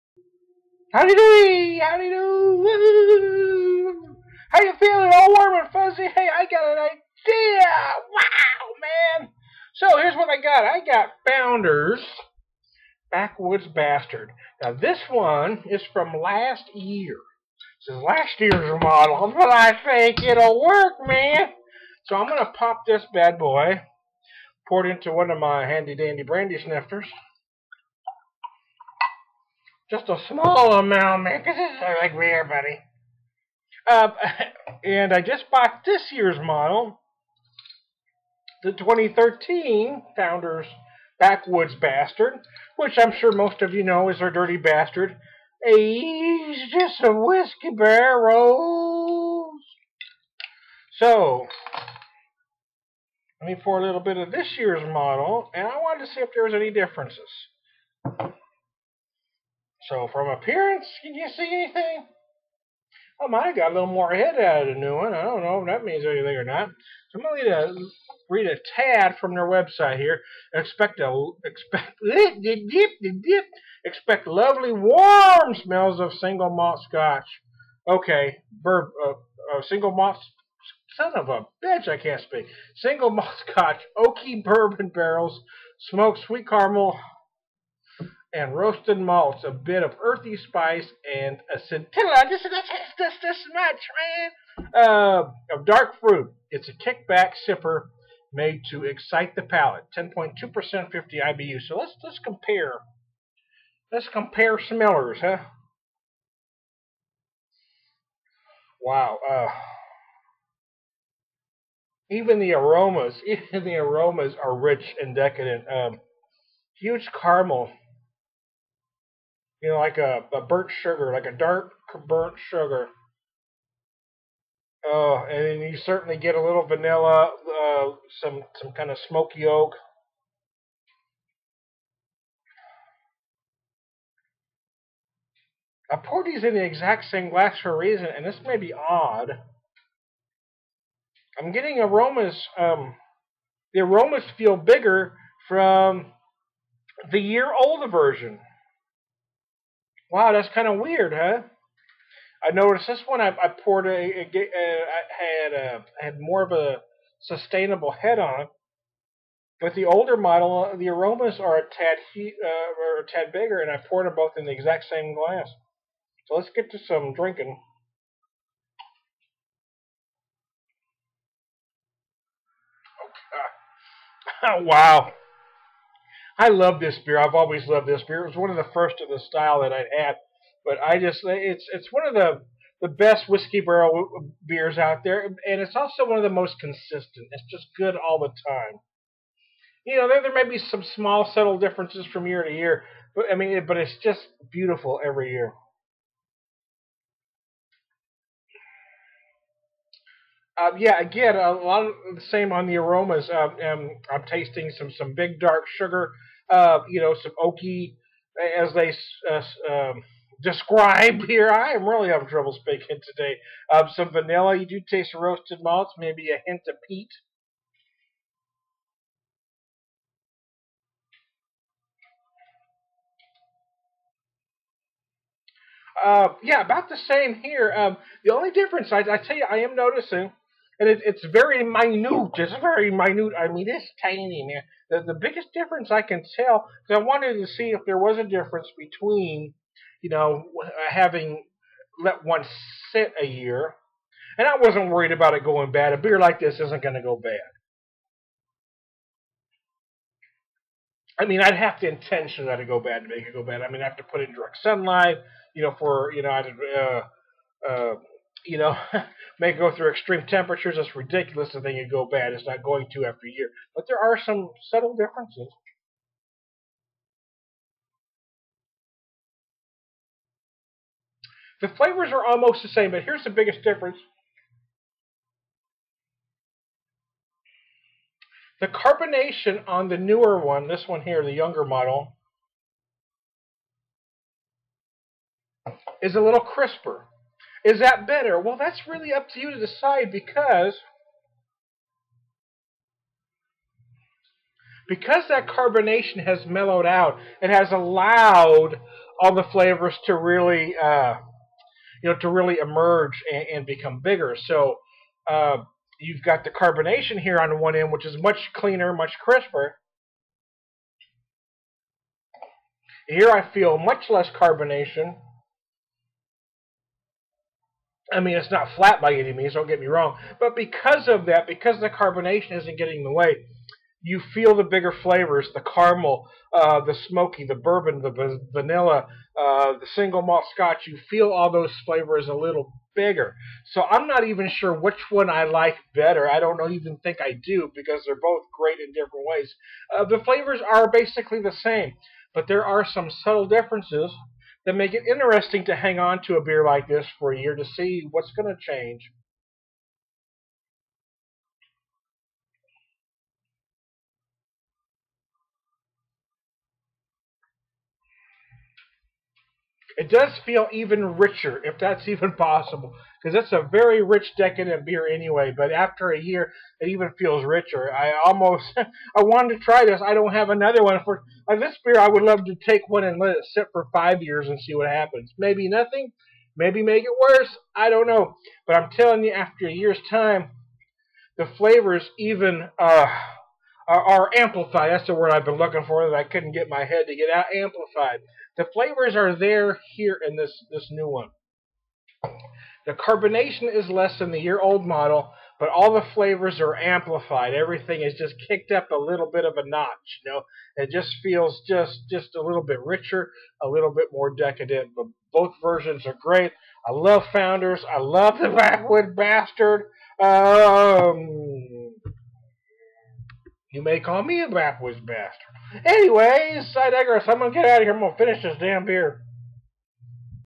Drinkin' With The Beer Whisperer: Battle Backwoods Bastards: 2012 VS.2013!
Founder's Backwoods Bastard, comparing the 2012, aged a year, to the 2013!